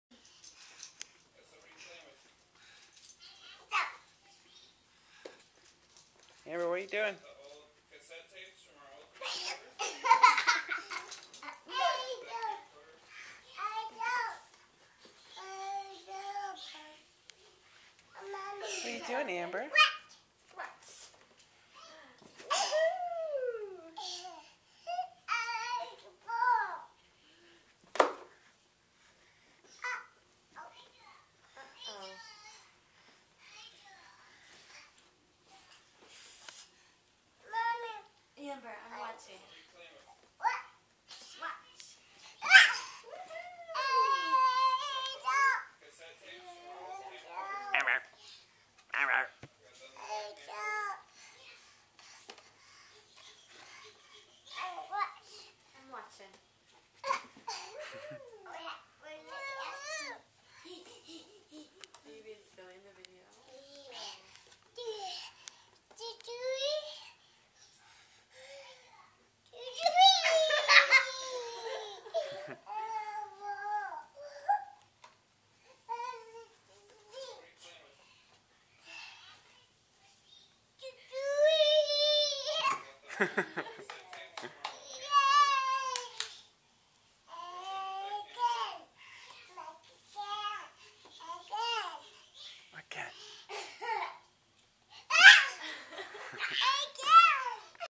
amber jumping
amber jumping off of a chair just for kicks... first time she's done this, and she's never seen anyone else do it